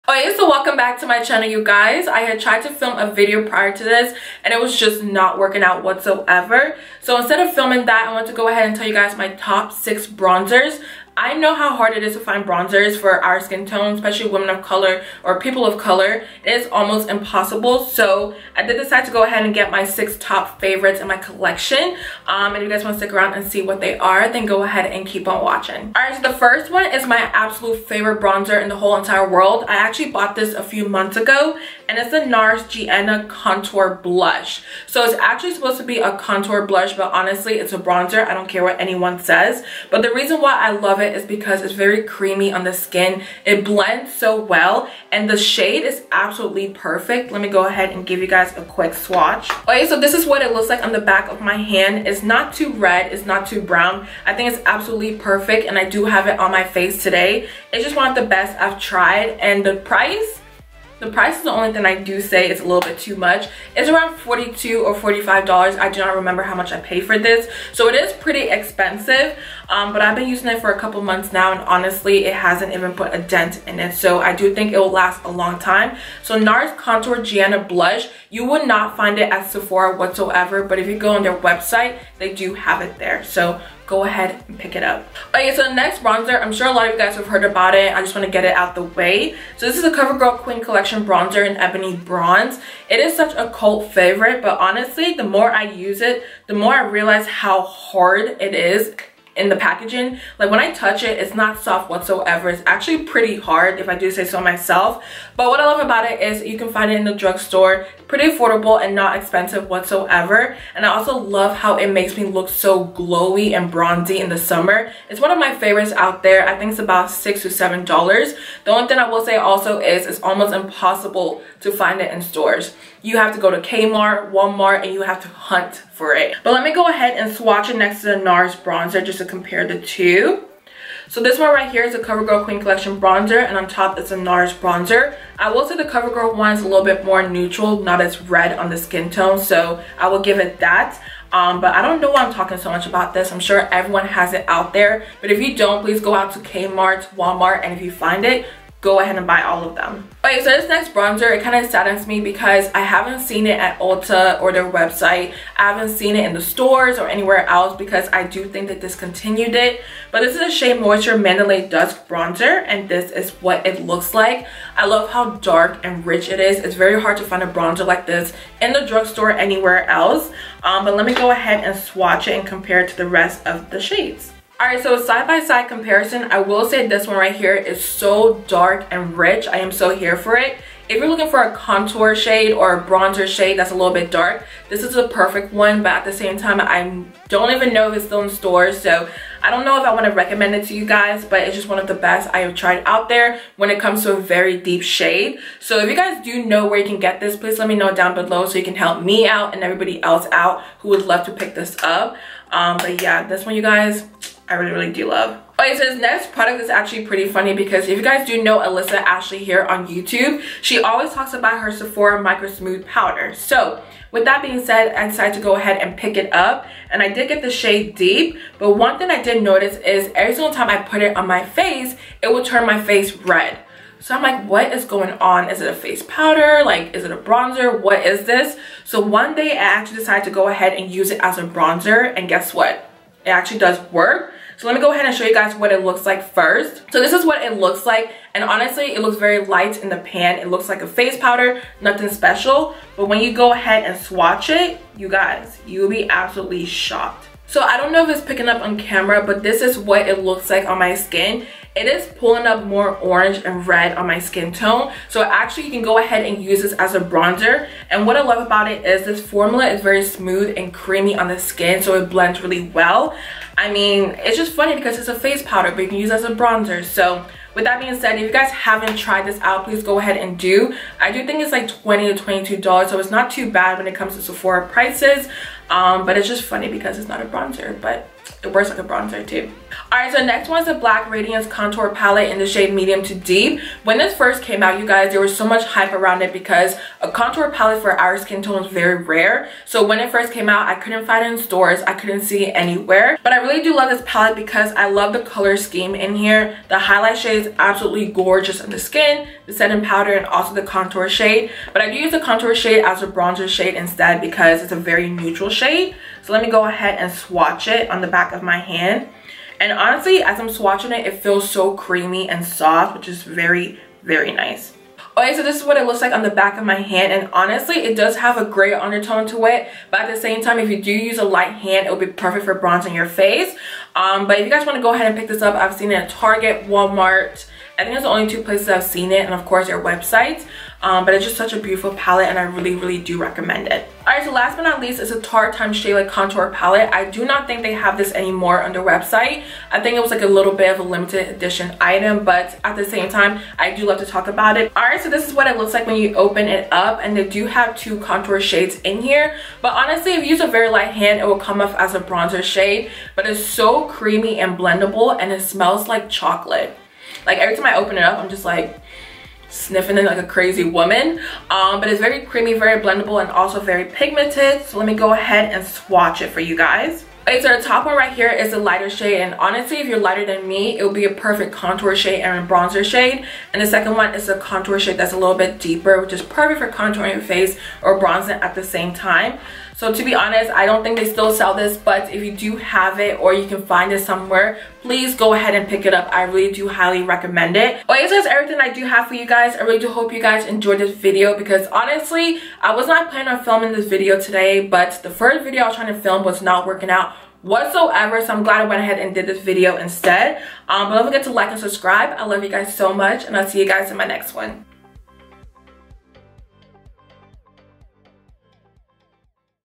My Top 6 Favorite Bronzers l Review + Swatches
Open..♡

What are some bronzers that you guys are loving? Let me know down below ♡

Nars Gienah Contour Blush
Covergirl Ebony Bronze Bronzer
Shea Moisture Mandalay Dusk Bronzer
Sephora Micro Smooth Powder Deep
Black Radiance Medium To Deep Contour Palette
Tarte x Makeup Shayla Contour Palette